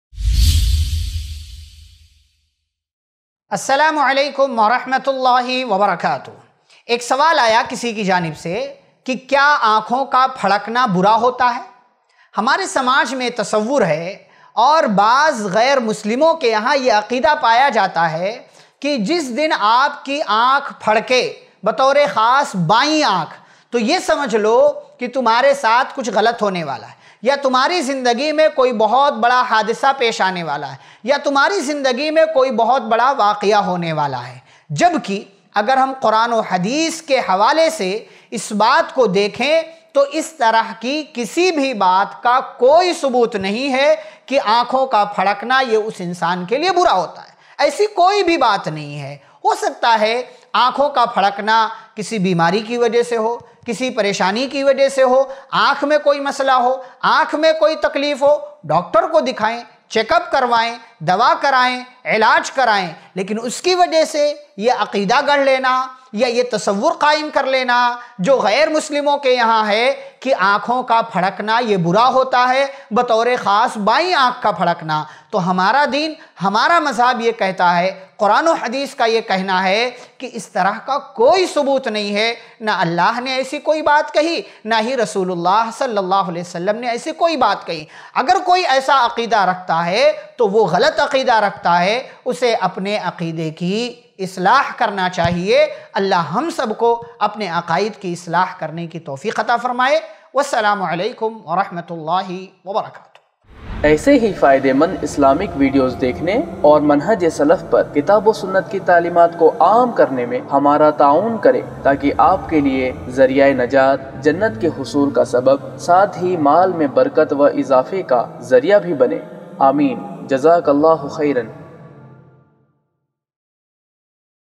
Kya Aankhon Ka Phadakna Bura Hota Hai Sawal O Jawab Series Ep.5 | Hafiz Khalil Ur Rahman Sanabili
Bank Account Details:
ILM FOUNDATION
ICICI Bank
Savings account
IFSC CODE-ICIC0001028

Kya Aankhon Ka Phadakna Bura Hota Hai Sawal O Jawab Series Ep.5 | Hafiz Khalil Ur Rahman Sanabili | IIC Mumbai Islam Helpline